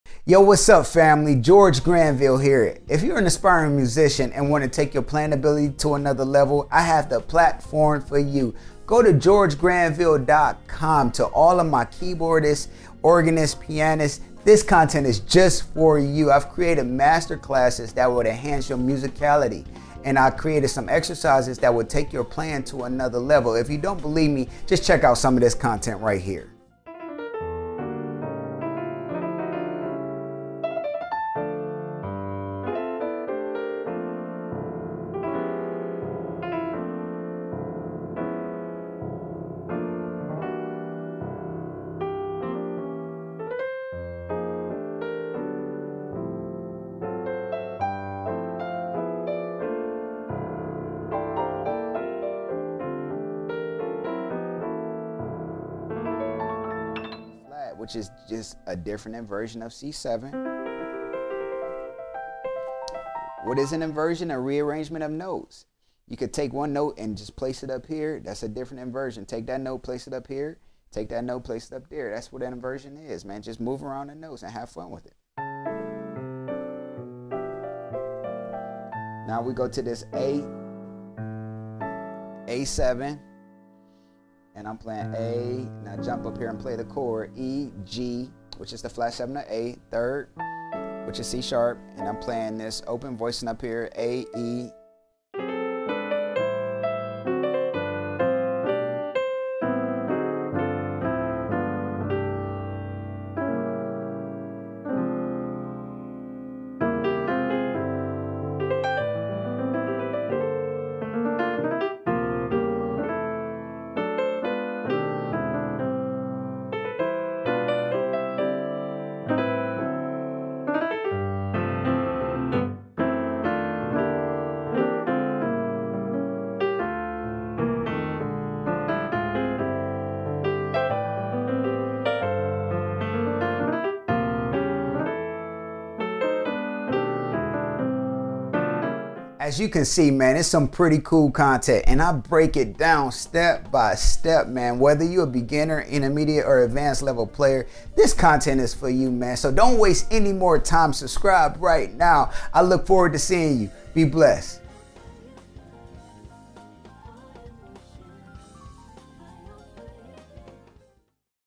Piano Masterclasses Available for Gospel & Jazz Musicians
Here’s where fun, creativity, and pleasure collide. This curated collection of courses will stretch your current skills to create new possibilities for self-expression and storytelling through your craft. Every class offers instruction and techniques for real-world application. Prepare to be both delighted and challenged as you become more fluent with each lesson. Learn Traditional & Contemporary Jazz and Gospel Techniques.
Go to GEORGEGRANVILLE.com NOW!